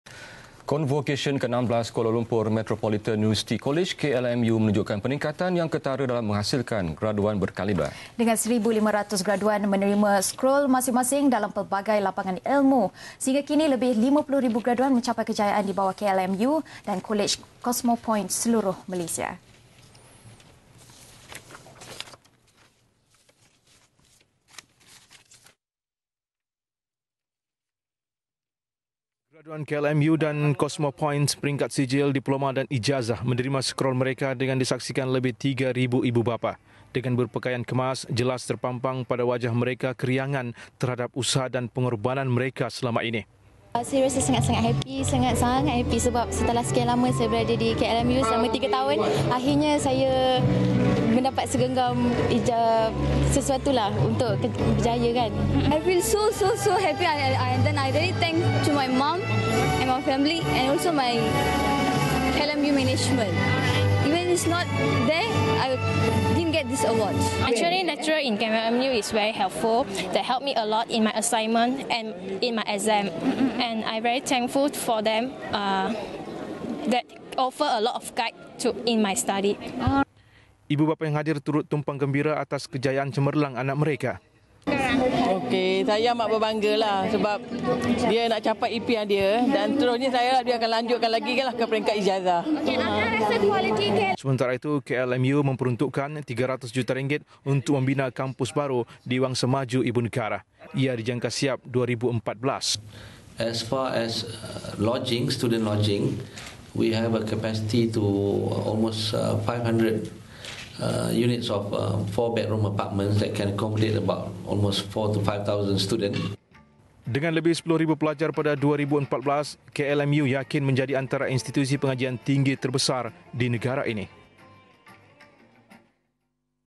16th Convocation KLMU
Datuk Idrus Mohd Satha
- Presiden KLMU

Izyan Syazwani Yaacob
- Diploma E-Perniagaan
Renuga Murthy
- Ijazah Sarjana Muda PEngurusan Perniagaan
Thong Shee Man
- Ijazah Sarjana Muda Pengurusan Pertadbiran